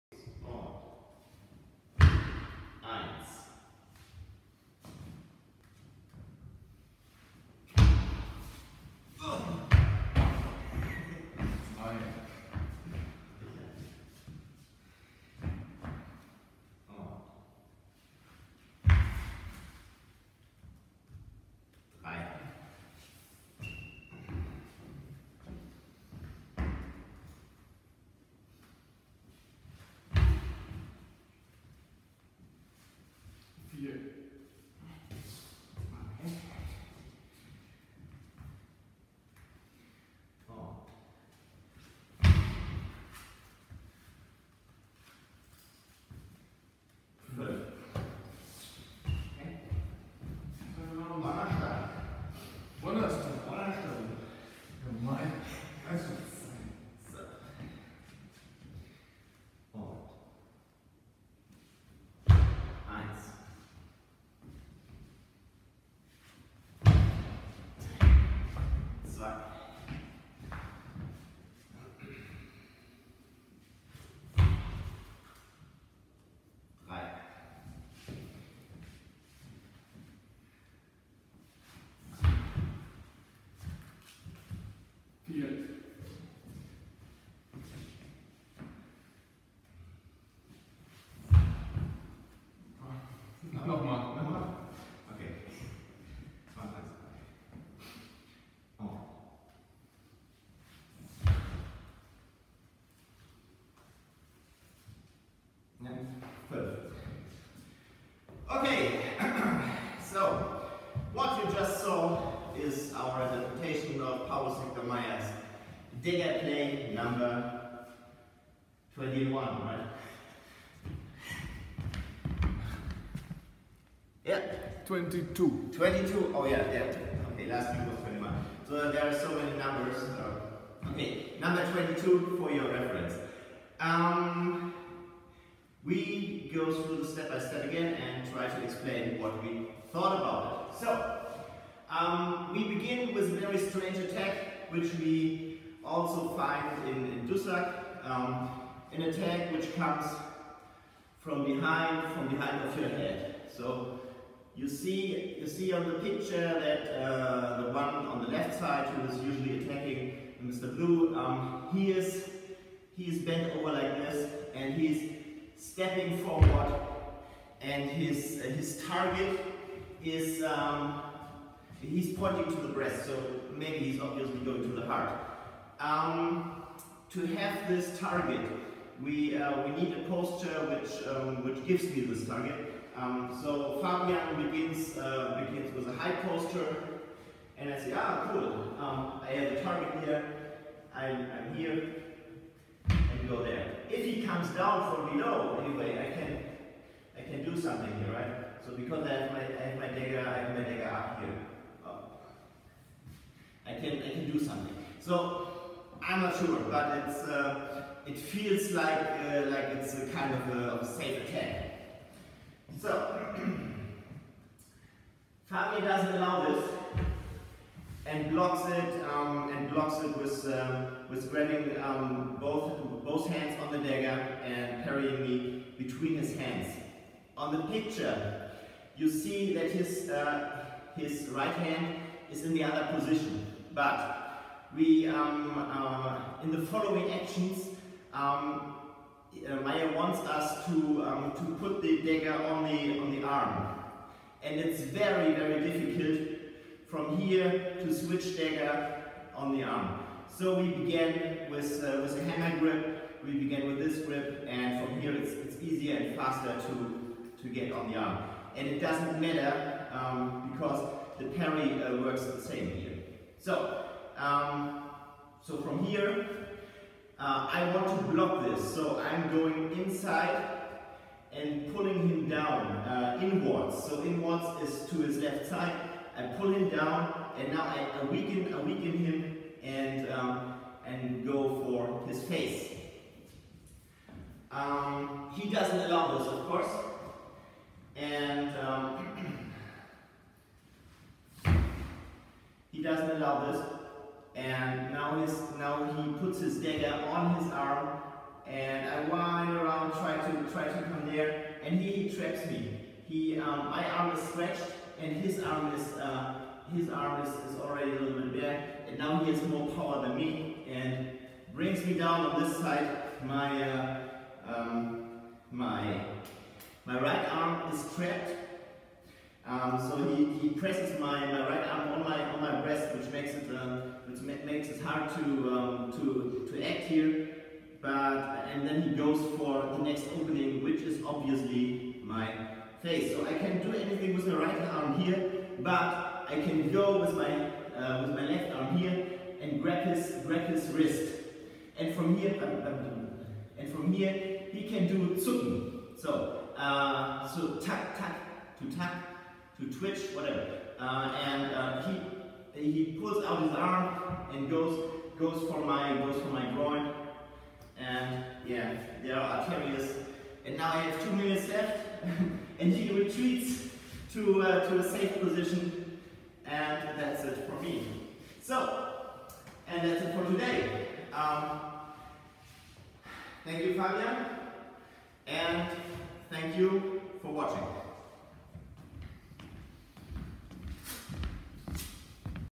Paulus Hector Mair - Historical Dagger fencing play No.22 - HEMA
learnhema .. This is an interpretation of dagger play No.22 from Paulus Hector Mair's
enormous compendium of martial arts teachings "Opus Amplissimum de Arte Athletica"
(MSS Dresd.C.93/C.94).

Dresden I Transcription (1540s) by Pierre-Henry Bas
Fencer: Fabian & Christian

---A breast thrust against a setting aside

Item: conduct yourself thus with this device in the onset. Wind yourself with stabs and with thrusts into him so that your right leg comes forward. Immediately wind your thrust behind your head to his breast [1].

If he should stab thus at you, so step in with your right leg and set it aside between both your hands on your dagger [2].

So if he has thus set your stab aside, so grip with your left hand toward the inside on his left arm, push strongly therewith under him on your right side. Immediately set your right leg back (we forgot this) and tug your dagger nimbly toward yourself. Immediately stab him in his face [3].

If he stabs thus at you so lay your dagger blade on your arm and take that away therewith. Immediately nimbly take two steps forward and seek the closest opening [4]. If he sets off this of yours so tug above and thrust below at him at his genitals [5]. Immediately set your left leg behind his right and rotate yourself therewith back away from him.